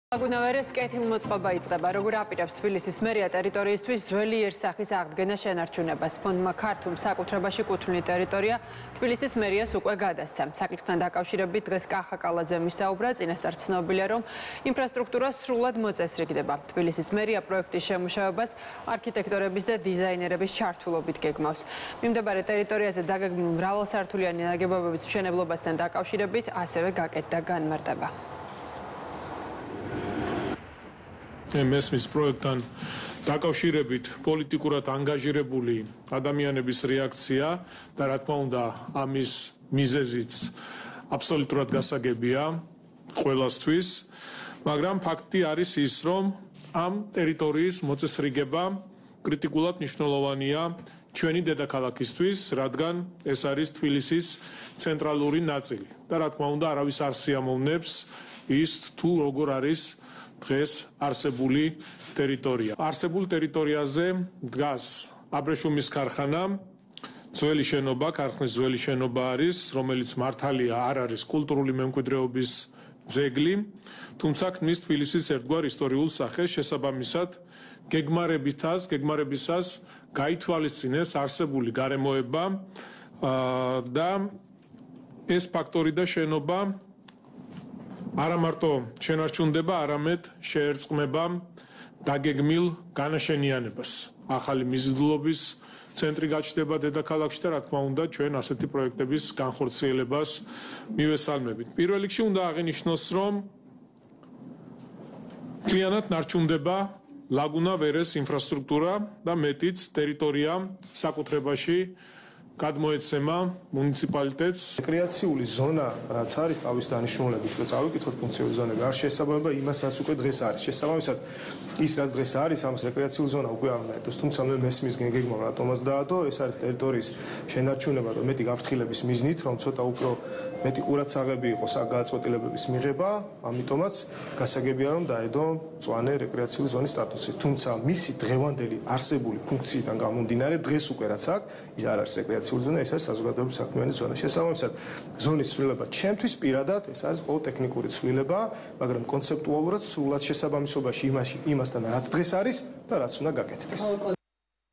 ახალი პროექტები თბილისში - სპეციალისტების გამოხმაურება
„ლაგუნა ვერეს“ კეთილმოწყობა იწყება - როგორ აპირებს თბილისის მერია ტერიტორიისთვის ძველი იერსახის აღგენა-შენარჩუნებას. „ფონდმა ქართუმ“ საკუთრებაში კუთვნილი ტერიტორია თბილისის მერიას გადასცა. საკითხთან დაკავშირებით, კახა კალაძემ ისაუბრა.

წინასწარ ცნობილია, რომ ინფრასტრუქტურა სრულად მოწესრიგდება. თბილისის მერია პროექტის შემუშავებას არქიტექტორებისა და დიზაინერების ჩართულობით გეგმავს. მიმდებარე ტერიტორიაზე დაგეგმილ მრავალსართულიანი ნაგებობის მშენებლობასთან დაკავშირებითაც გაკეთდა განმარტება.